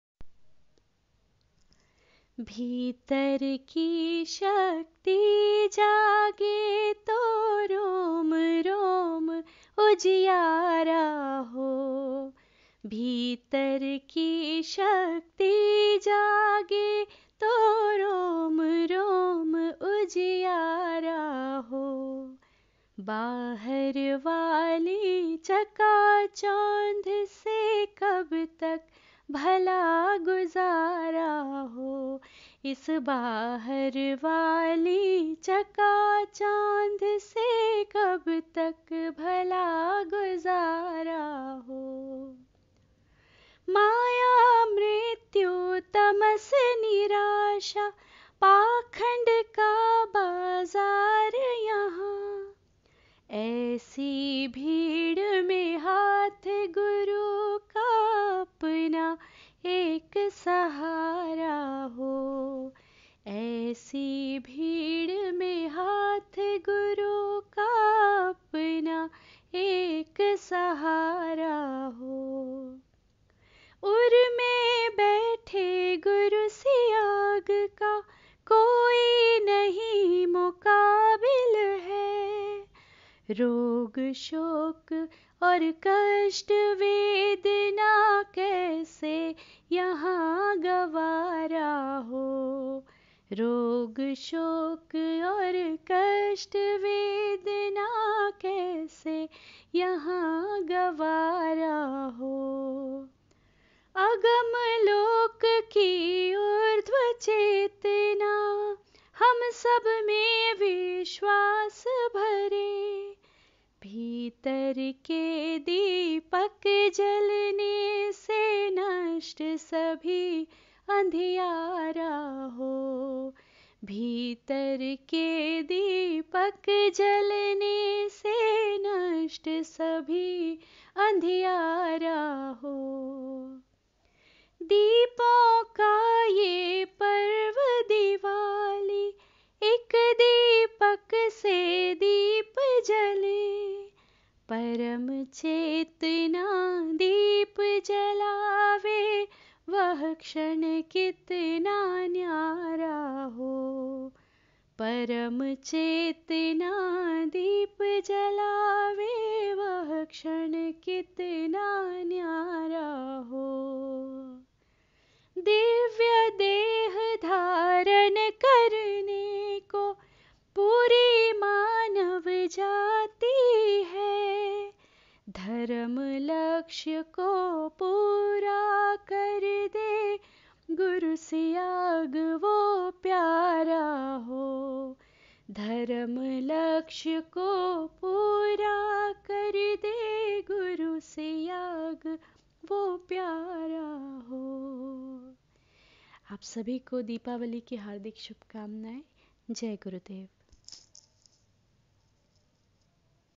गुरु सियाग योग - दीपावली की हार्दिक शुभकामनाएं।
आप सभी को दीपावली की हार्दिक शुभकामनाएं। इस पावन अवसर पर गुरुबहन राखी उपाध्याय द्वारा एक भजन को स्वर दिये गए हैं। गुरुदेव के पवित्र चरणों को कोटि कोटि प्रणाम। सभी को बहुत बहुत जय गुरुदेव!!!